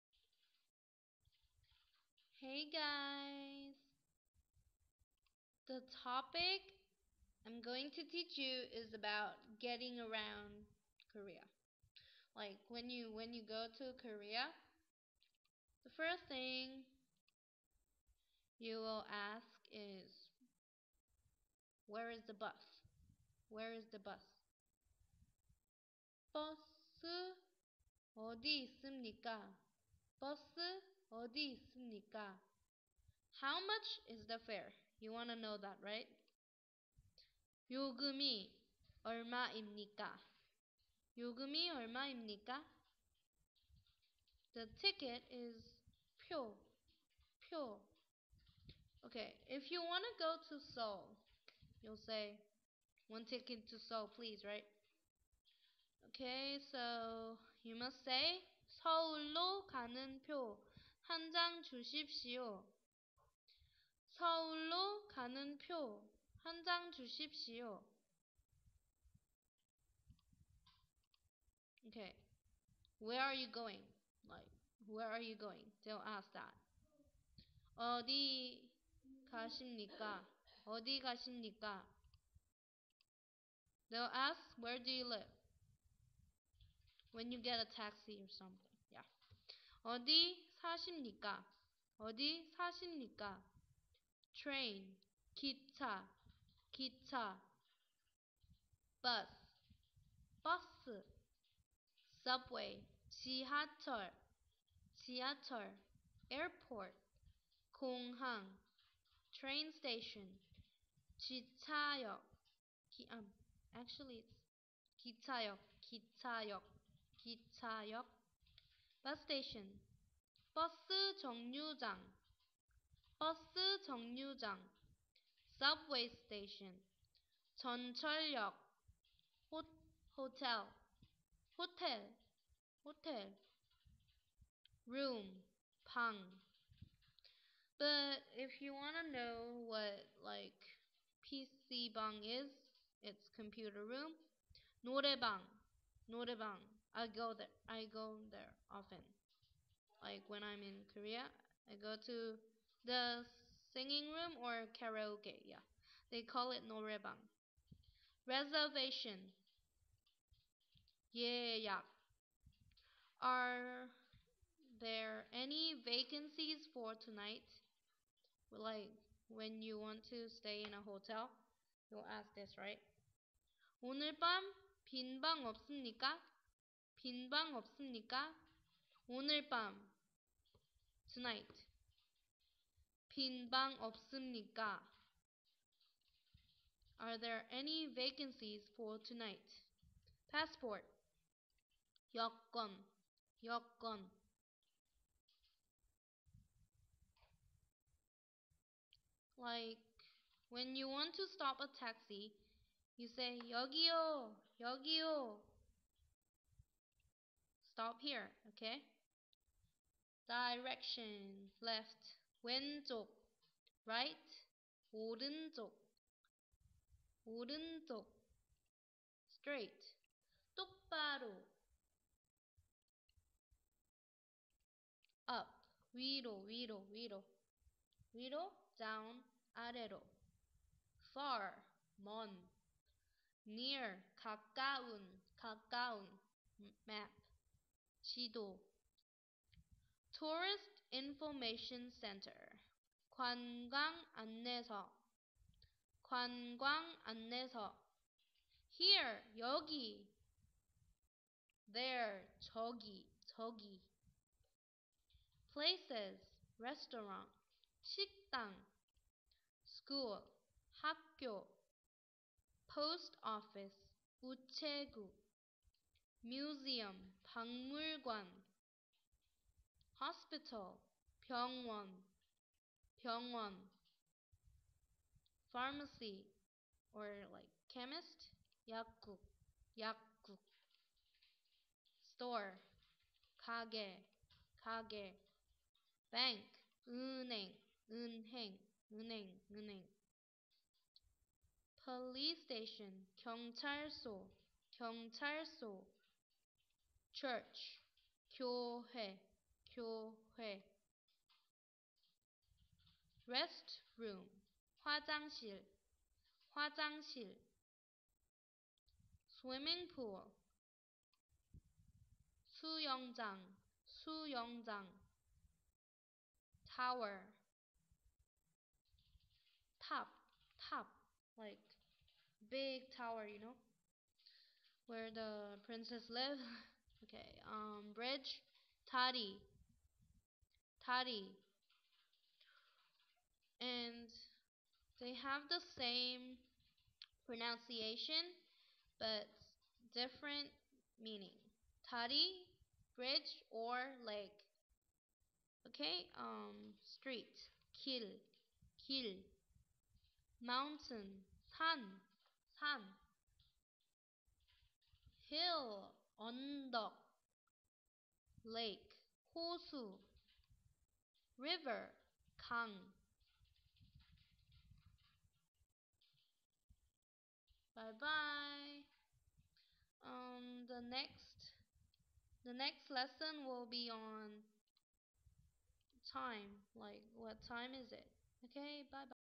Getting Around Korea
Where is [the bus]? : [Bus]-i eo-di-e i-seup-ni-ka

How much is the fare? : yo-geum-i eol-ma-im-ni-ka

One ticket to [Seoul] please : (Seoul-ro) ga-neun pyo han-jang ju-sip-si-yo

Where are you going? : eo-di-e ga-sim-ni-ka

Where do you live? : eo-di-e sa-sim-ni-ka

train : gi-cha

bus : bus

subway : ji-ha-cheol

airport : gong-hang

train station : ji-cha-yeok

bus station : bus jeong-geo-jang

subway station : jeon-cheol-yeok

hotel : hotel

room : bang (PC Bang-computer room) (Norae-bang-singing room)

reservation : ye-yak

Are there any vacancies for [tonight]? : [o-neul-bam] bin-bang eop-seum-ni-ka

passport : yeo-kwon

Stop here : yog-ee-o (to stop a taxi)

Directions
  left : wen-chok

right : o-reun-chok

straight : ddok-ba-ro

up : wi-ro

down : a-re-ro

far : meon

near : ga-ka-un

map : ji-do

tourist information center(office) : gwan-gang an-ne-so

here : yyog-ee

there : chog-ee

 Places
  Restaurant : sik-dang

School : hak-gyon

Post office : u-che-guk

Museum : bak-mul-kwan

Hospital : byeong-won

Pharmacy, Chemist : yak-guk

Bank : eun-haeng

Police station : gyong-chal-seo

Church : gyo-hye

Restrooms : hwa-jang-sil

Swimming Pool : su-yeong-jang

Tower : tap

Bridge : da-ri

Street : gil

Mountain : san

Hill : eon-deok

Lake : ho-su

River : gang